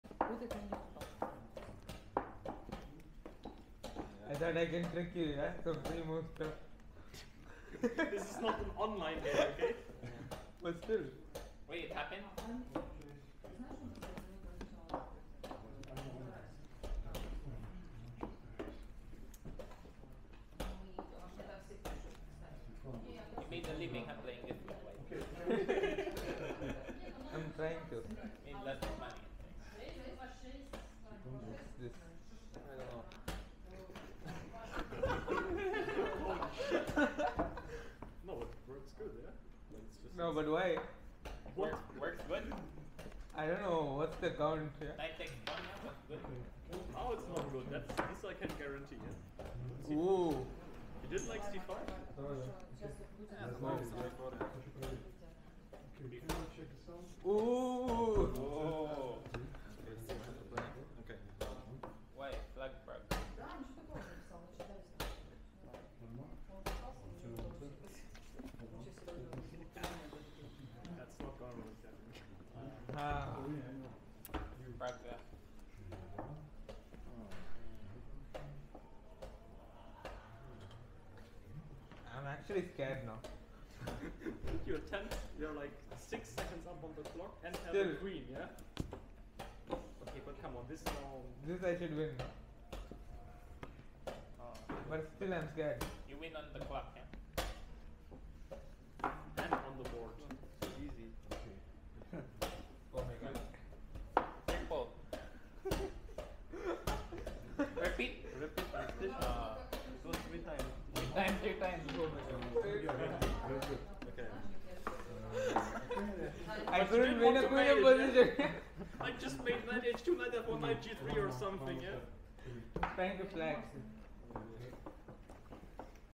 I have Queen and I'm scared! Praggnanandhaa vs Duda
Bullet games between world best players are mesmerazing! Rameshbabu Praggnanandhaa played several games with Jan-Krzysztof Duda. Polishman during FIDE World Rapid Team Championship after three wins in a row against Indian players was granted with title "Indian Destroyer". Somehow Pragg was able to revenge his countrymen.